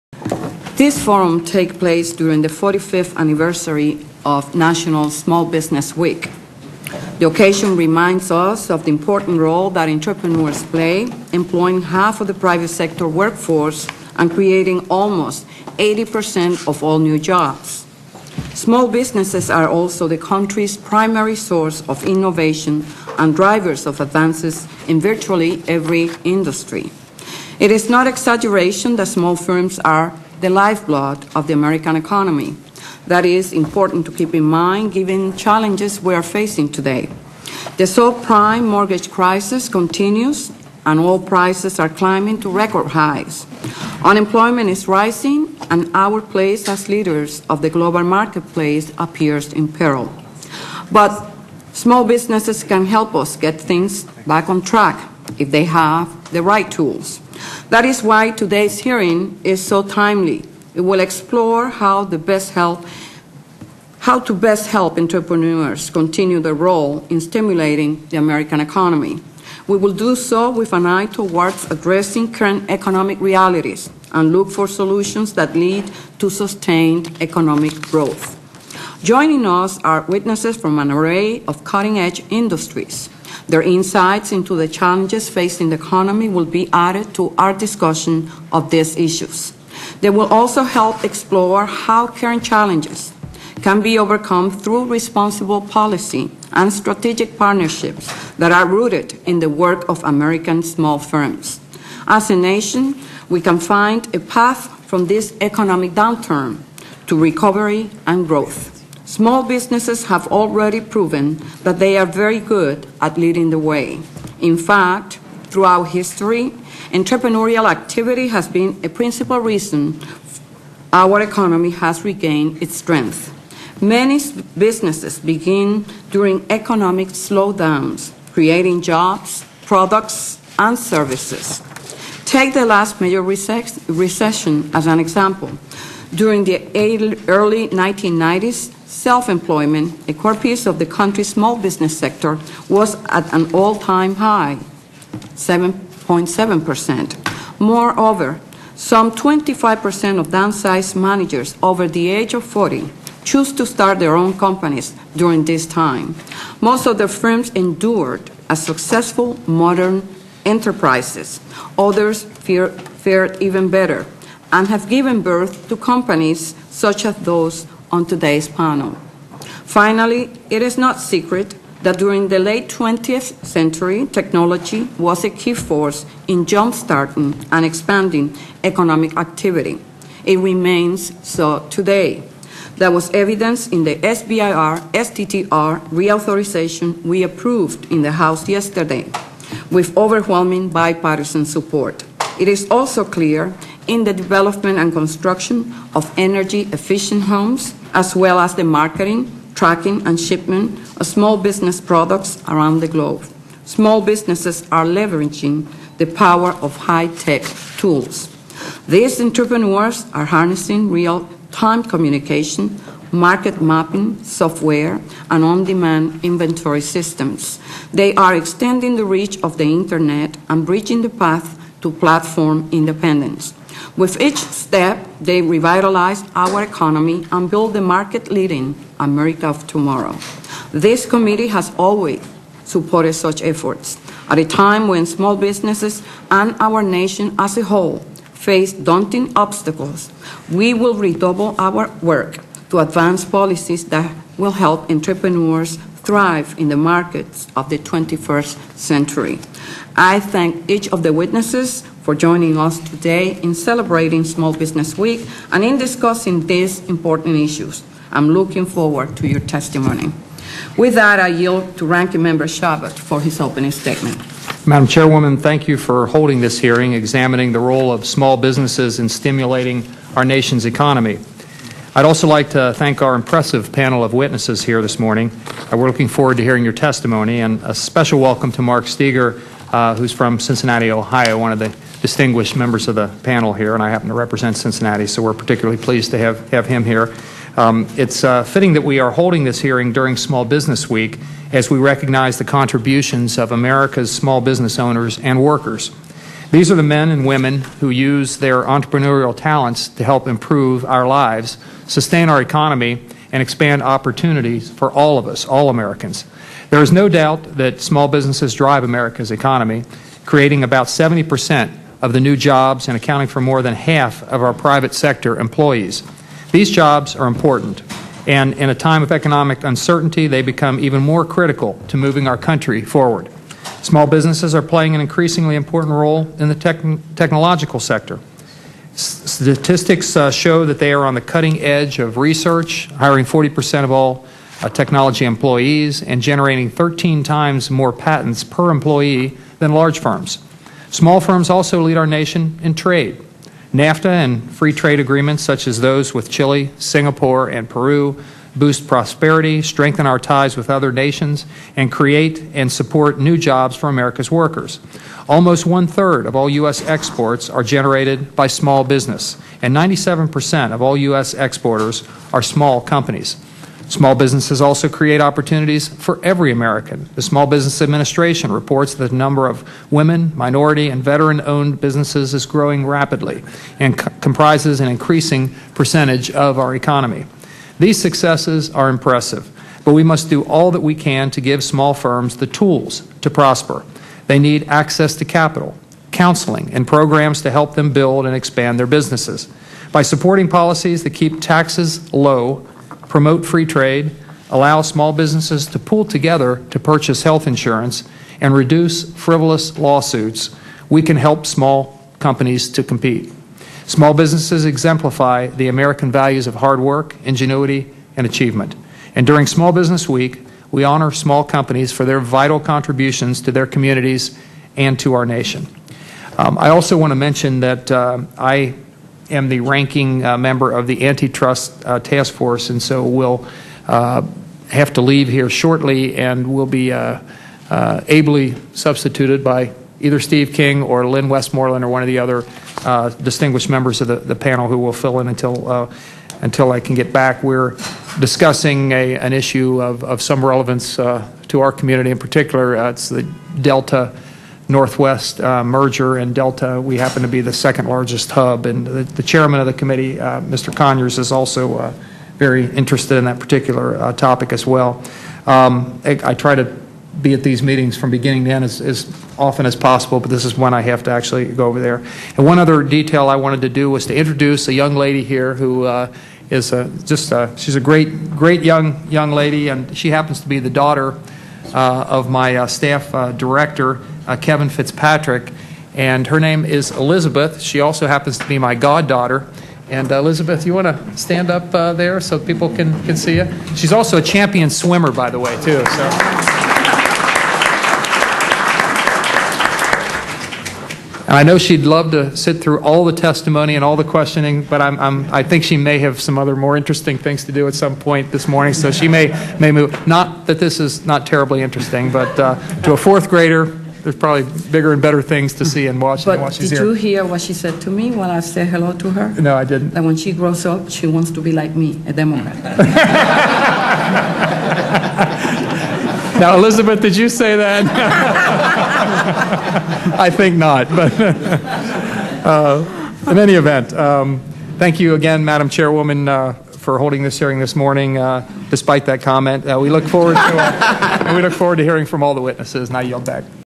Role of Small Businesses in Stimulating the Economy - 1
Opening statements by Chairwoman Nydia Velázquez (D-NY) and Ranking Member Steve Chabot (R-OH)